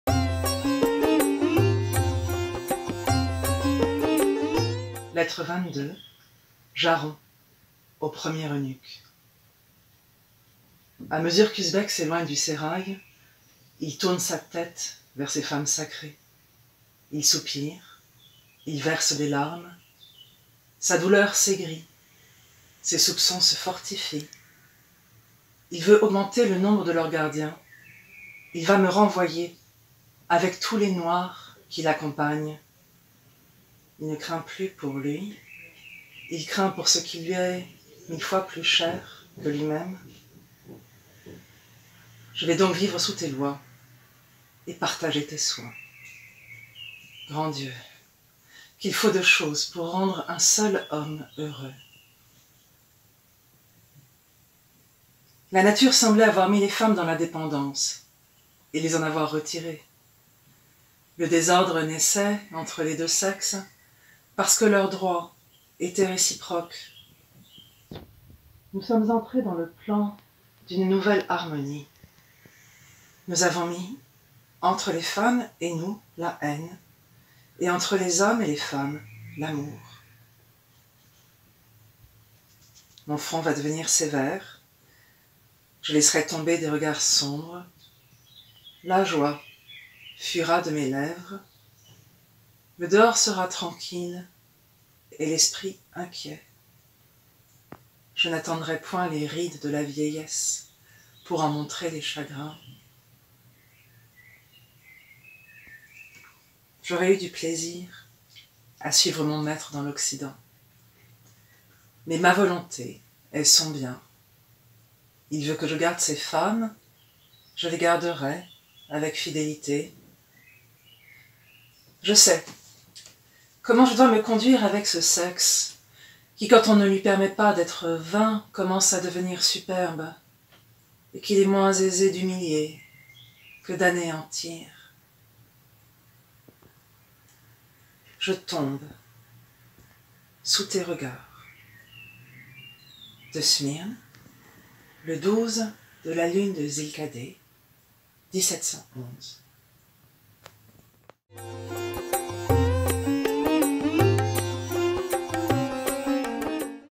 Lettres persanes #22 par Gwenaëlle Aubry
Alors que la France de 1721 subissait une épidémie de peste menant la moitié Sud à des mesures drastiques de confinement, Charles-Louis de Montesquieu publiait au printemps ses Lettres persanes. Ces écrits sont un instantané éclairant la société de l’époque et dont les lumières brillent encore aujourd’hui. Une centaine d’artistes, poissons pilotes du théâtre, lisent jour après jour ces 161 lettres.